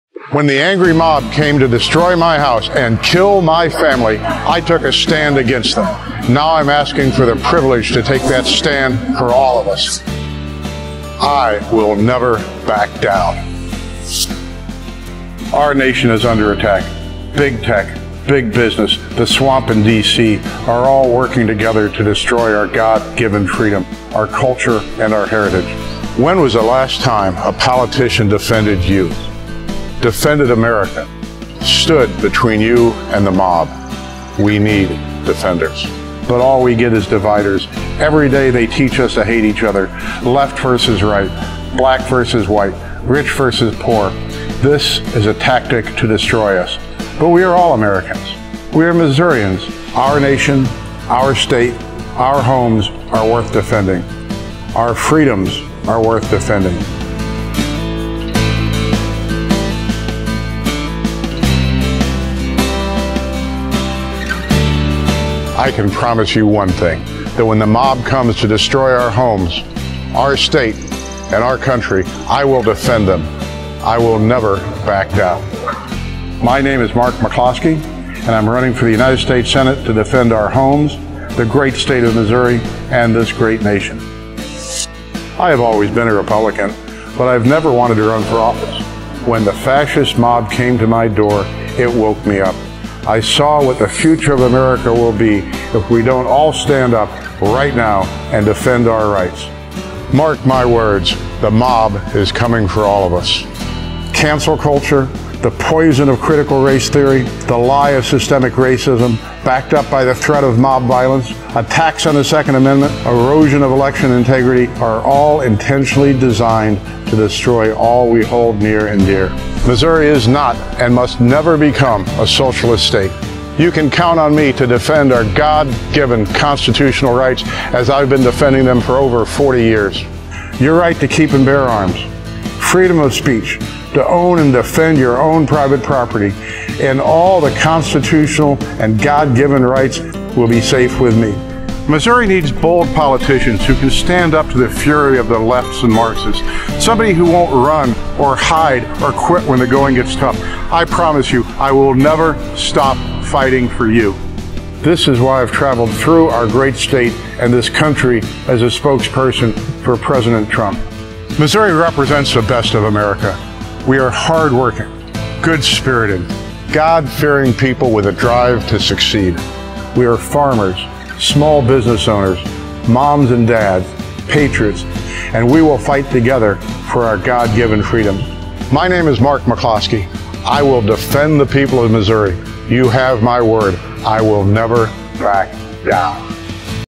Mark McCloskey Announces Senate Run With An America First Platform
Mark McCloskey's video has been going viral all over social media and Americans know this guy is not afraid to stand up for the people of Missouri!